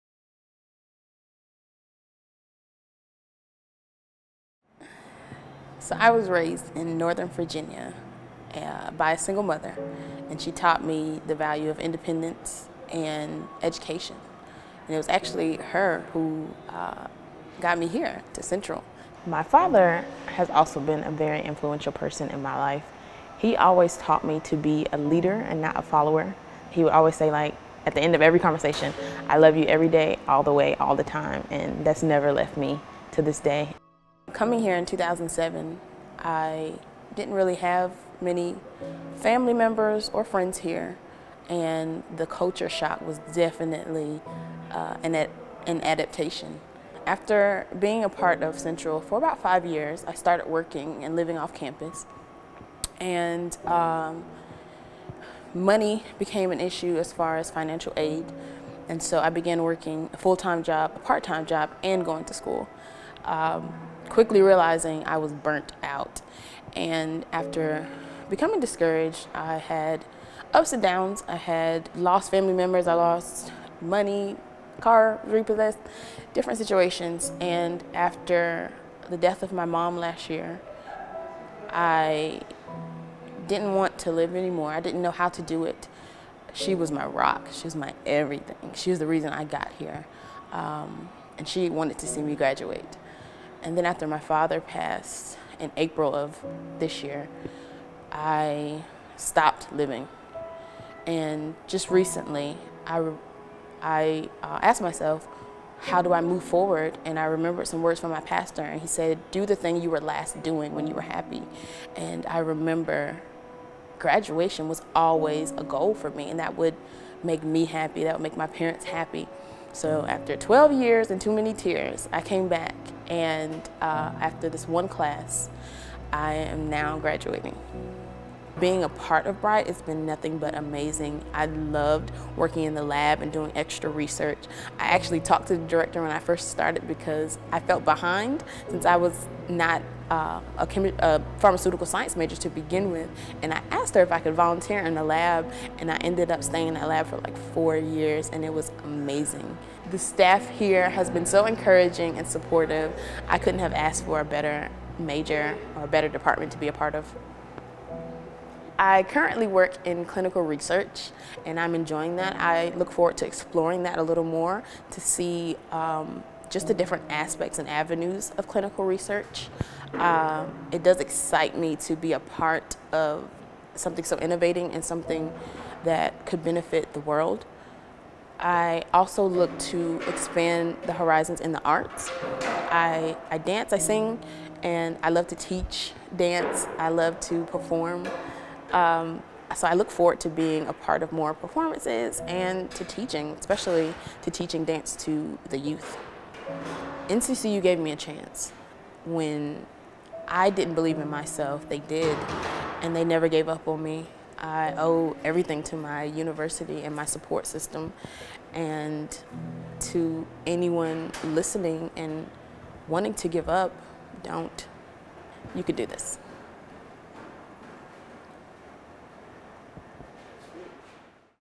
Kendra Davis, '19, B.S., Pharmaceutical Sciences
Students undergo many hardships during their college years, including financial stresses and unforeseen family issues.
Both came at once for graduating senior Kendra Davis.

Listen to her journey of perseverance and triumph.